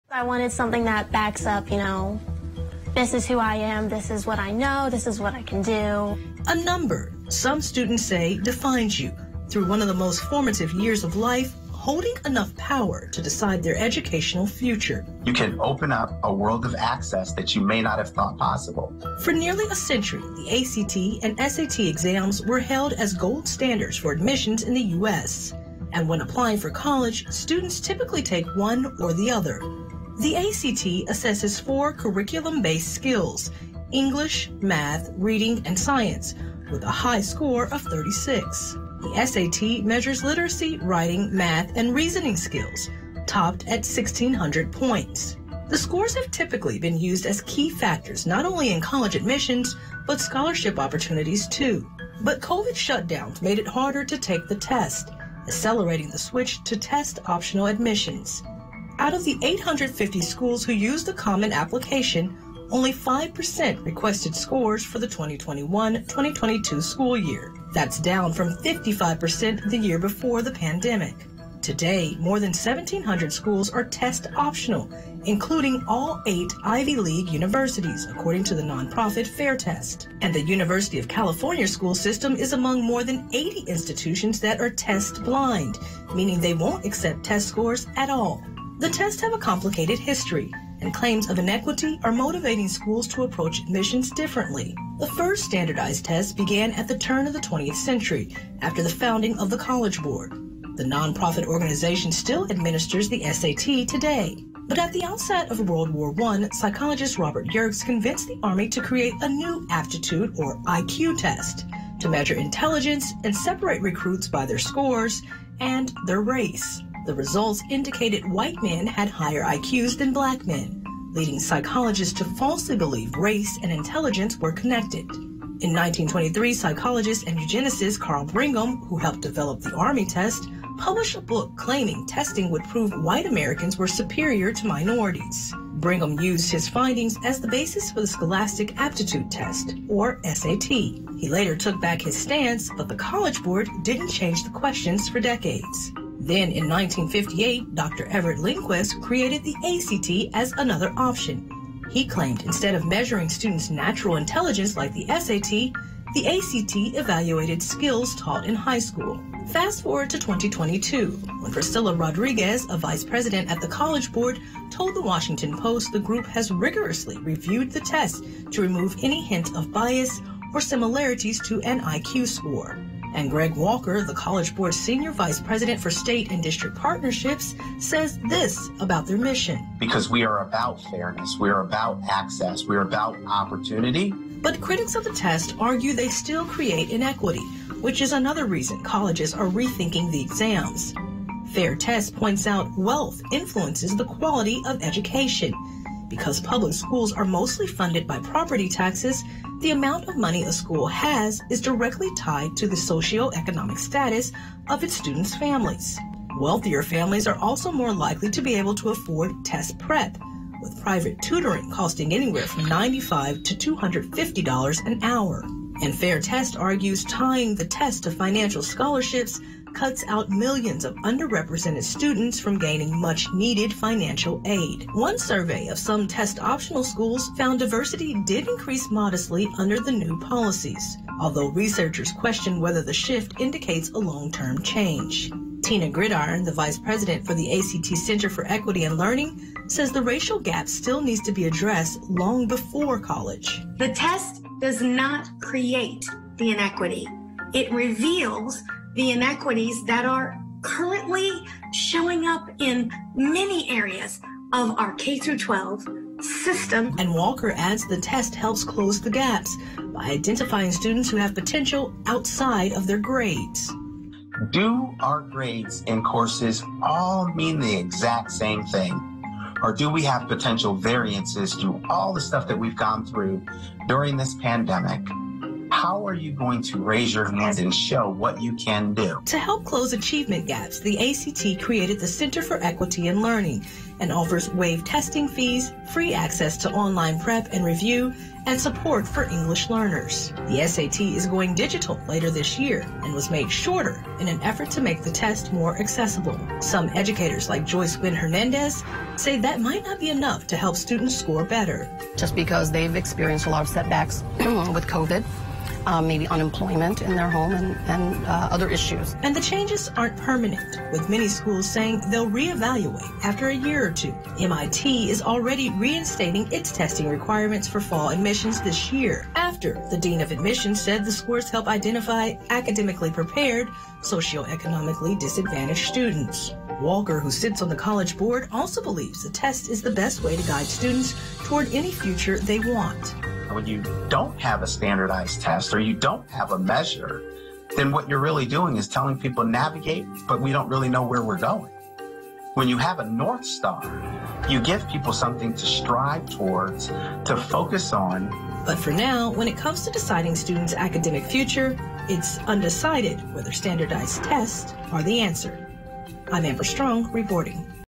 Why Some Schools Are Rethinking Standardized Tests?
For nearly a century the ACT and SAT exams were hailed as gold standards for admissions in the U.S.— and when applying for college, students typically take one or the other. The scores have typically been used as key factors not only in college admissions, but scholarship opportunities, too. But COVID shutdowns made it harder to take the tests, accelerating the switch to test-optional admissions.